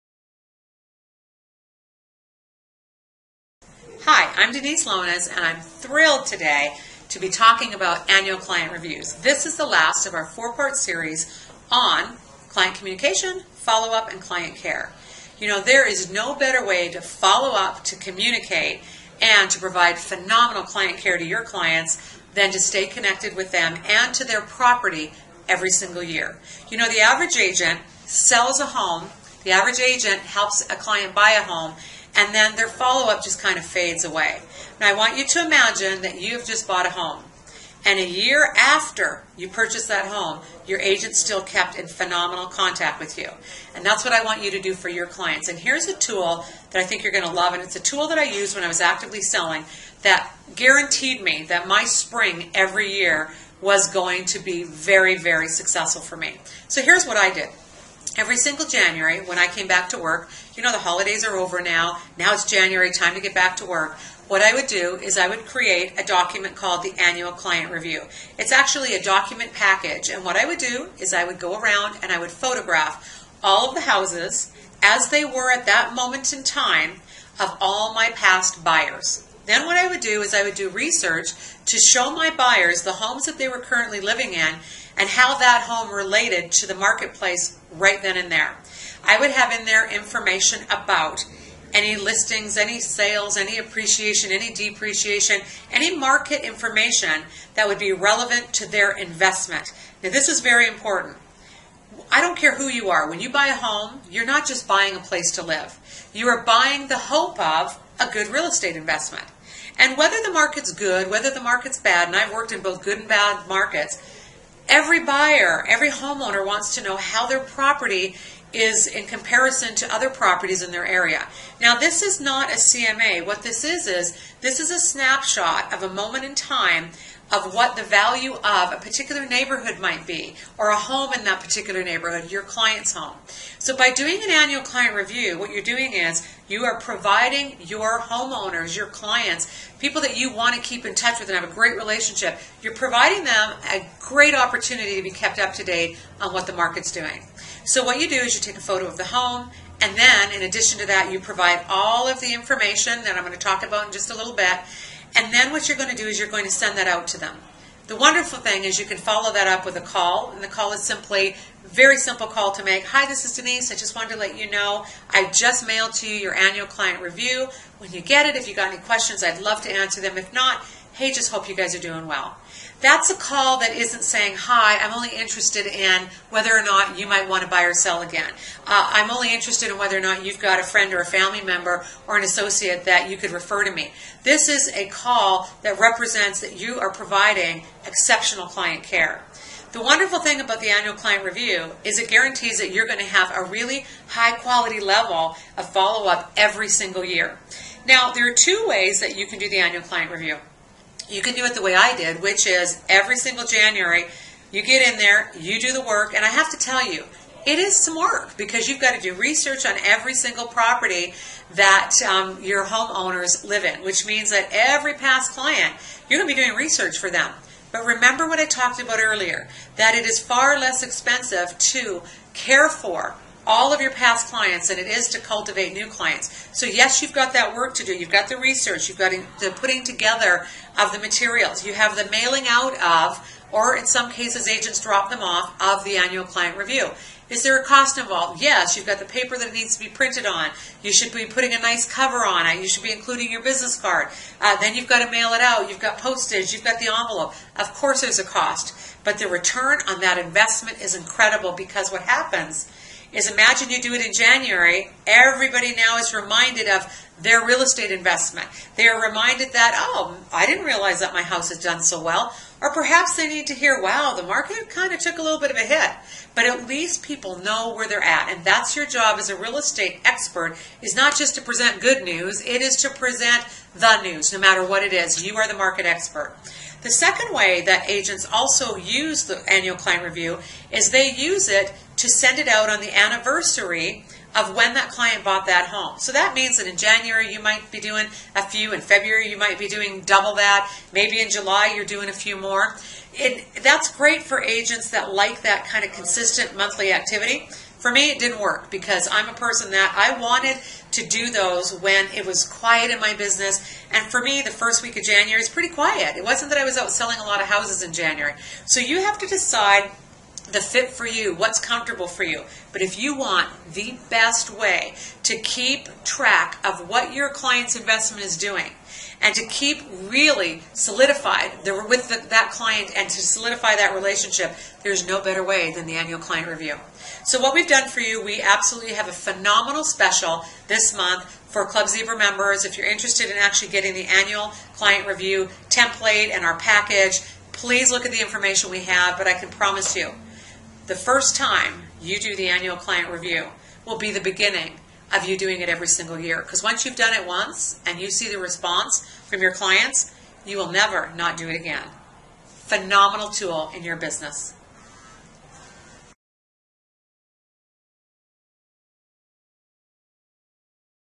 Follow-up Series, Part 4: Annual Client Review
Annual Client Reviews allow you to continue building relationships with past clients by delivering a snapshot summary of their home's value. The fourth and final video in our follow-up and client communication series discusses annual client reviews, and how to implement them in your business.

Learn more about this series and other real estate focus topics on our